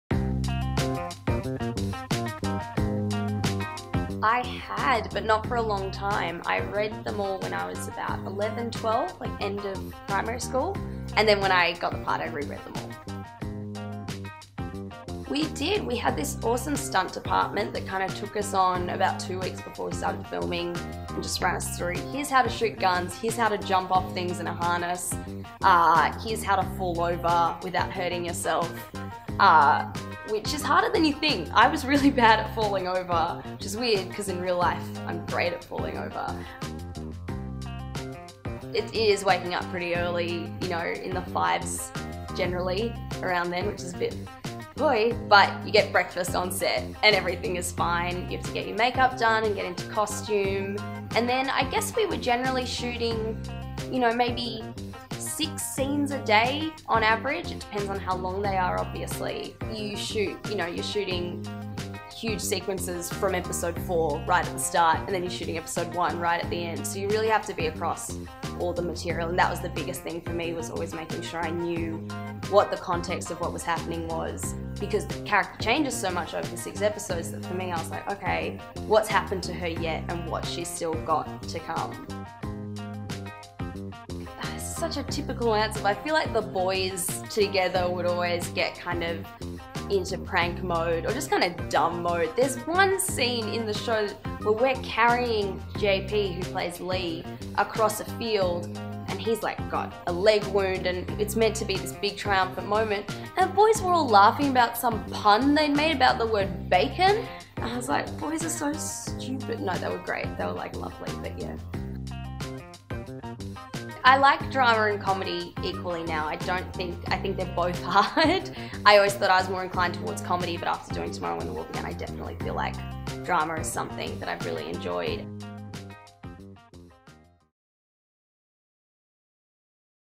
Tomorrow When The War Began - Molly Daniels Interview
Tomorrow When The War Began:
Based on the era-defining novels by John Marsden, Tomorrow, When the War Began – the Series, tells the story of a group of teenage friends who, upon their return from a remote camping trip find themselves thrust into the middle of a war no one saw coming.